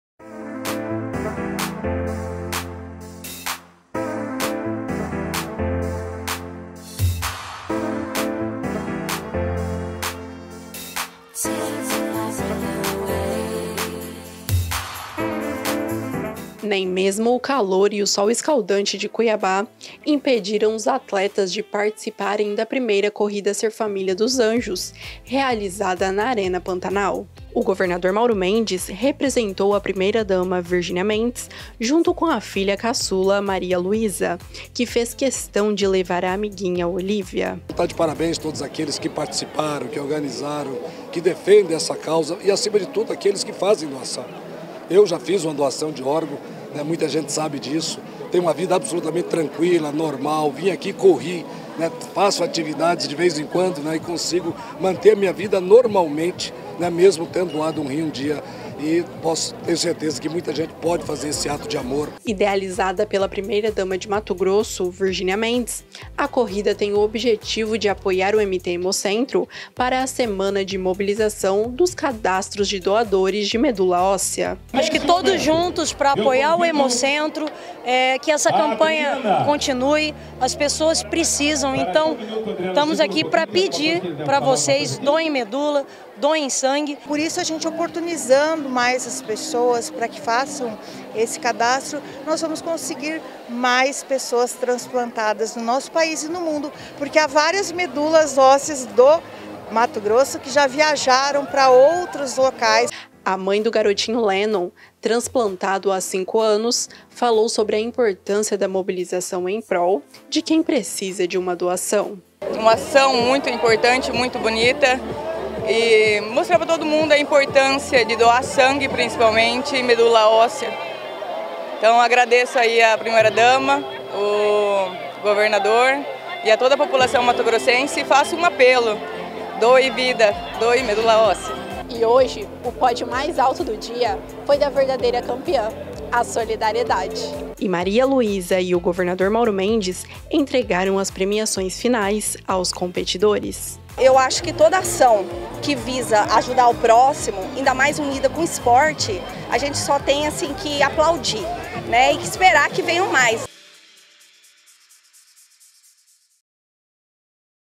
Idealizada pela primeira-dama de MT, Corrida Ser Família dos Anjos reúne atletas em mobilização
A 1ª Corrida SER Família dos Anjos foi realizada neste sábado (20.05), na Arena Pantanal, e antecedeu a semana de mobilização dos cadastros de doadores de medula óssea. A campanha de Cadastro Voluntário de Medula Óssea, este ano, ocorre entre os dias 22 e 26 de maio, na sede do MT Hemocentro, com diferentes atividades. Além de idealizadora, a primeira-dama de Mato Grosso é madrinha da corrida.